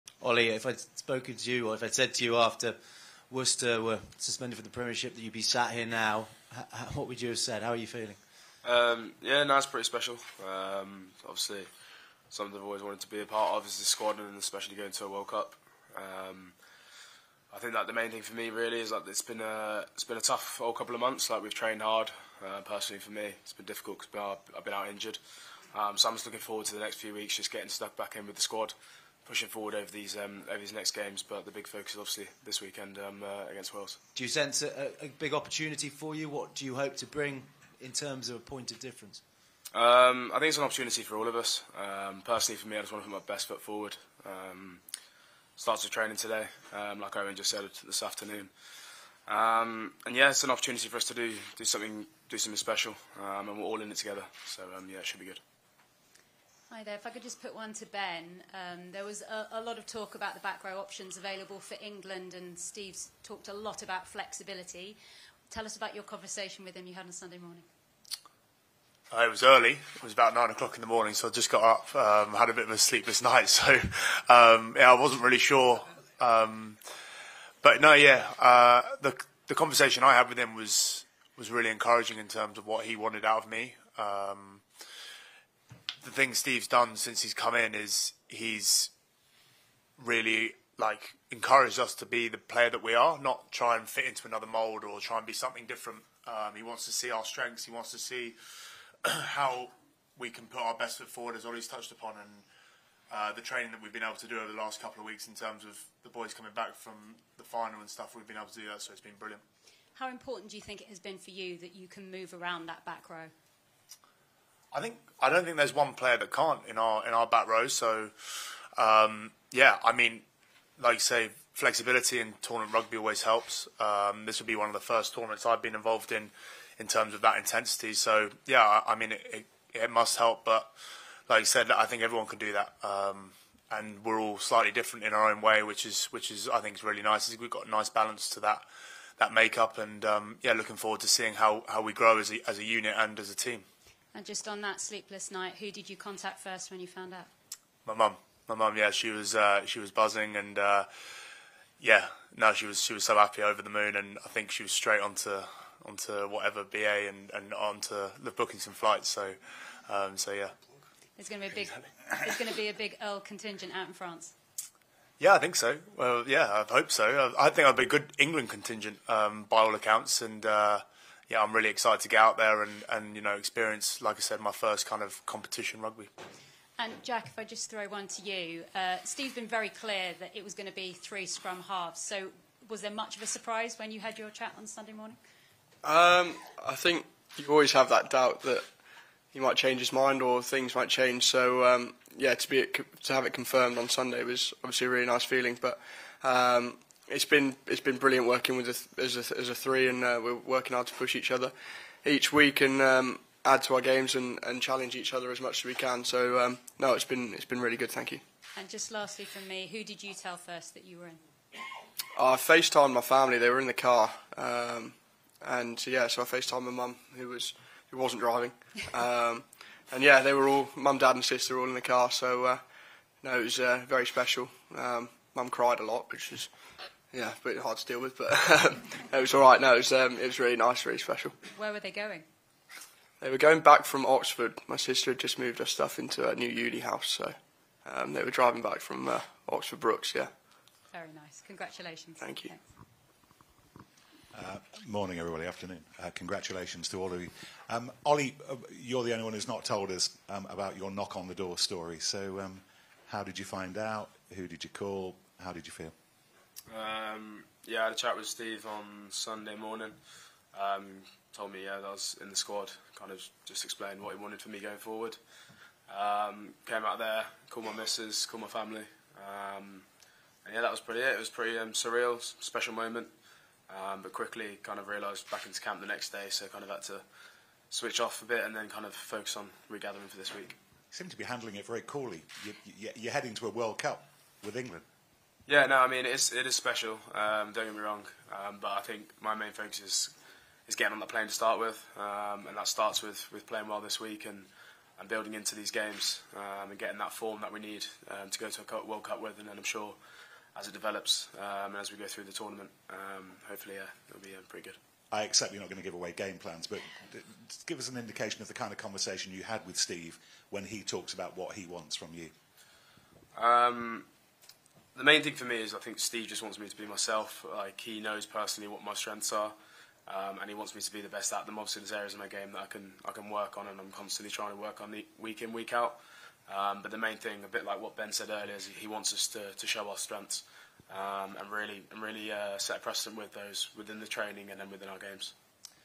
Ollie Lawrence on his insane journey that has led to selection for England's Rugby World Cup squad
From being unemployed 10 months ago to going to the Rugby World Cup in France Ollie Lawrence has had quite the journey. Jack Van Poortvliet and Ben Earl speak on their selection as well as they prepare to take on the challenge.